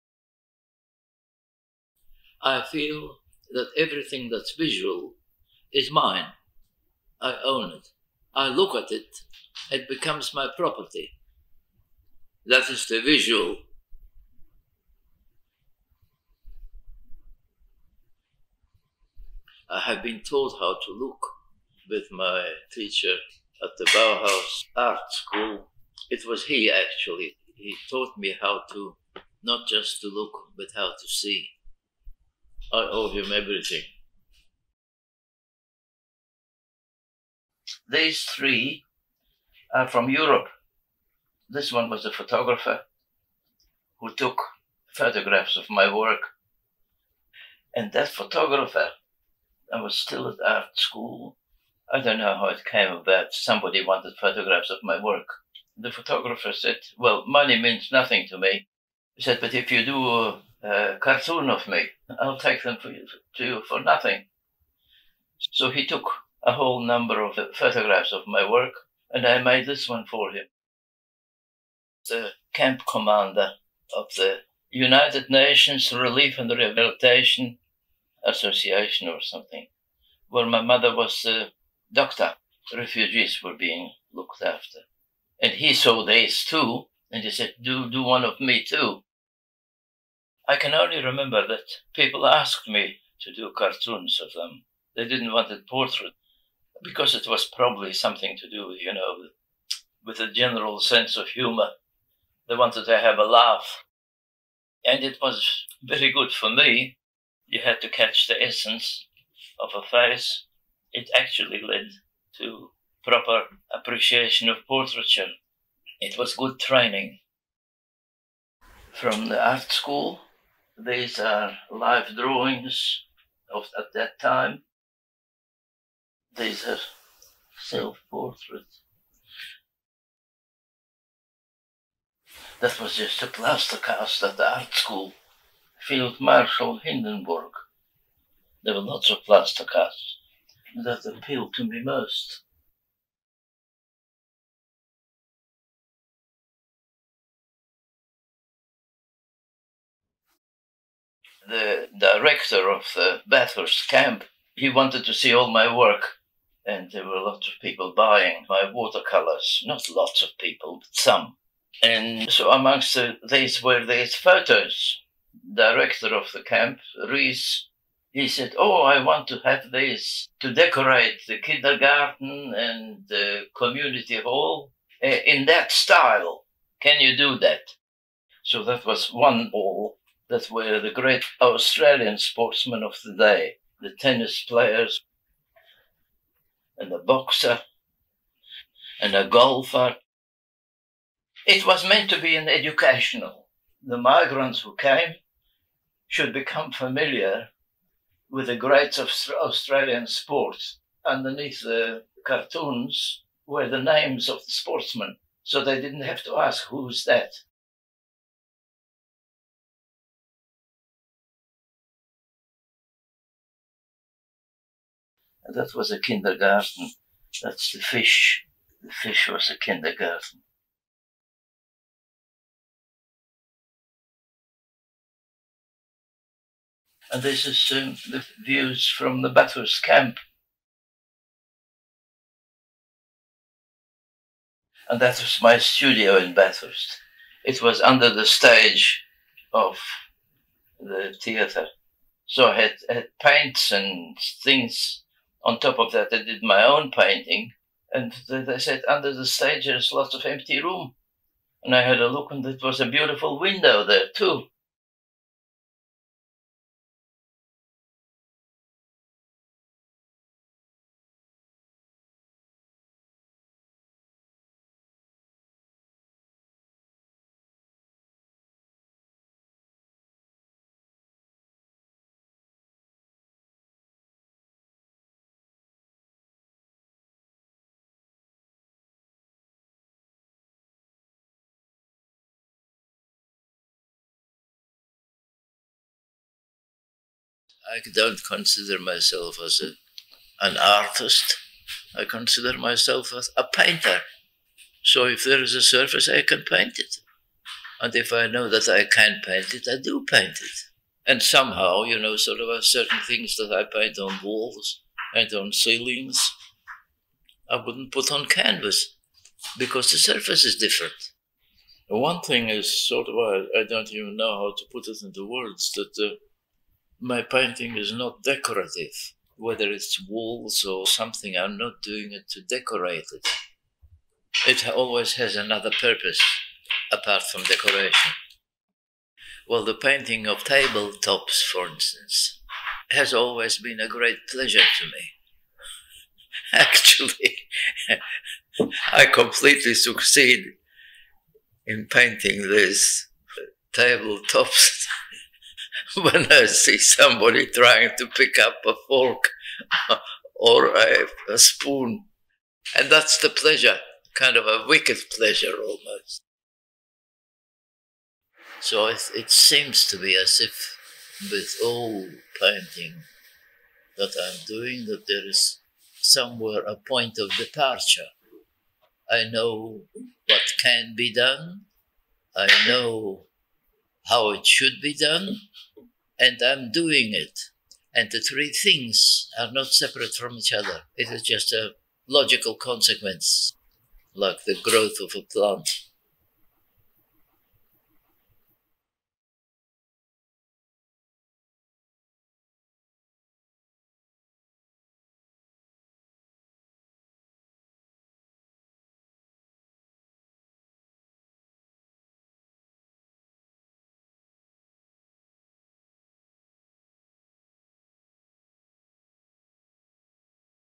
Jurgis Miksevicius: In the Light of the Sun and Shadow of the Moon
12 December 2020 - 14 February 2021
A BRAG exhibition

Lithuanian-born Jurgis Miksevicius (1923 – 2014) arrived in Australia aged 25 on the SS Charlton Sovereign with 725 other Baltic displaced emigres on 29 October 1948. Before leaving Germany, Miksevicius had studied at a Bauhaus-style art school in Darmstadt.

On arrival Miksevicius was settled at the Bathurst Migrant Camp but unlike most refugees who were transitioned out of the migrant camp after a few weeks, Miksevicius stayed for nine months. The camp director, William Rees, was aware of Miksevicius’ artistic skills and employed him to decorate the walls of the former army barracks’ recreation hall with caricatures of great Australian sportsmen, including swimmer John Marshall and tennis champion John Bromwich. Miksevicius was also provided with a studio space and encouraged to paint. It was here, in the Bathurst Migrant Camp, that he began to experiment painting with oils and trying to come to terms with the light, the gum trees and the Bathurst landscapes. Essentially, it was here in Bathurst that he began his 70-year career as an artist in Australia.

This exhibition Jurgis Miksevicius: In the light of the sun and shadow of the moon charts his development as a painter: his studies at art school in Germany, his early paintings and murals at the Bathurst Migrant Camp, and then focuses on later landscapes and philosophical ‘moon’ paintings as examples of the many genres he explored throughout his creative life.

Video copyright: Carolyn Leigh, Helena Miksevicius
Writer and producer: Carolyn Leigh
Photography: National Gallery of Australia, Canberra, Rodney Weidland, Graeme Wienand
Editors: Michelle Murch, Joel Tonks